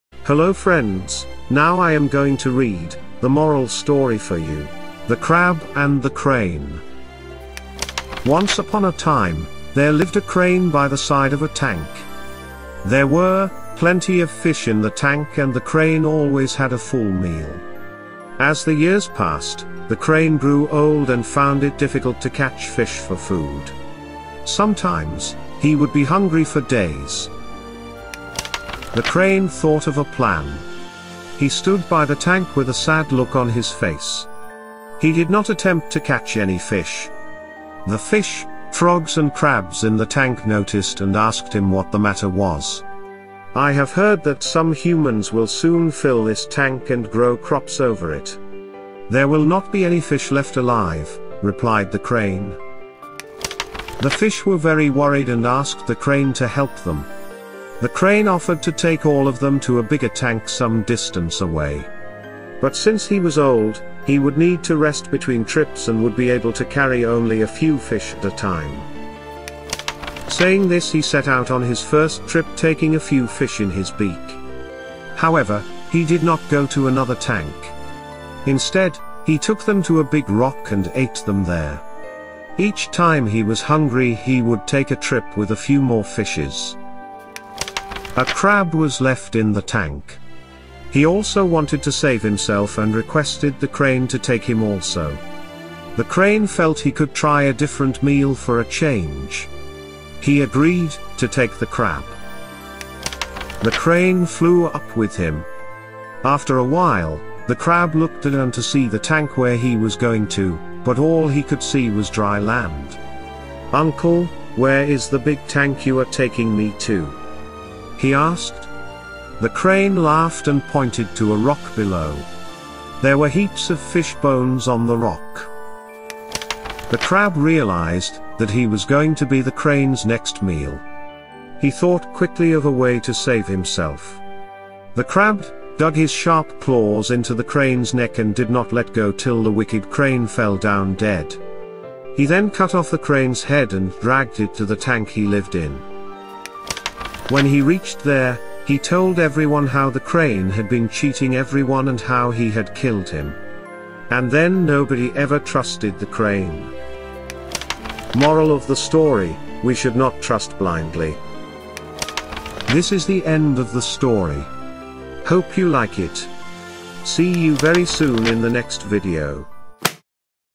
The Crab and The Crane - Bedtime Story - Read Short Story
This is the short story: The Crab and The Crane is one of the best collections of short stories we read it for you. These moral stories is a must for everyone who wants to learn some good habits or wisdom.